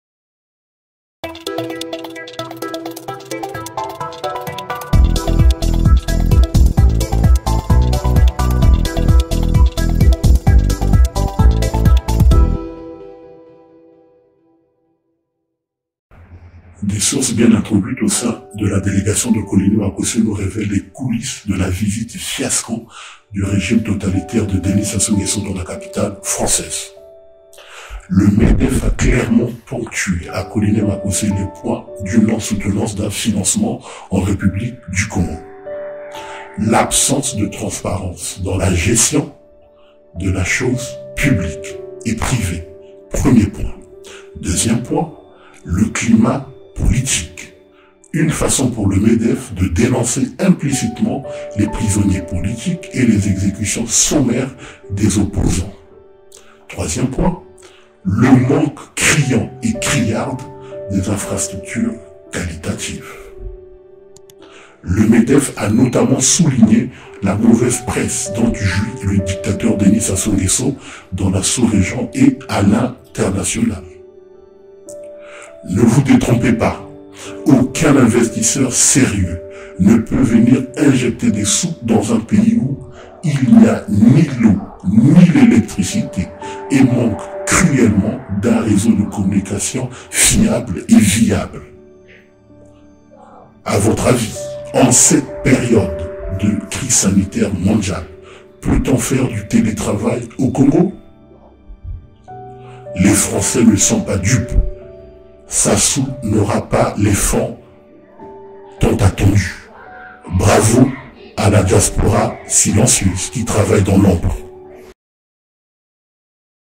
LE MEDEF DIT NIET AU DICTATEUR DENIS SASSOU NGUESSO
Le MEDEF dit niet au Dictateur Denis Sassou Nguesso pour un soutien financier.

DEVENIR MEMBRE

NOUS SUIVRE

NOUS CONTACTER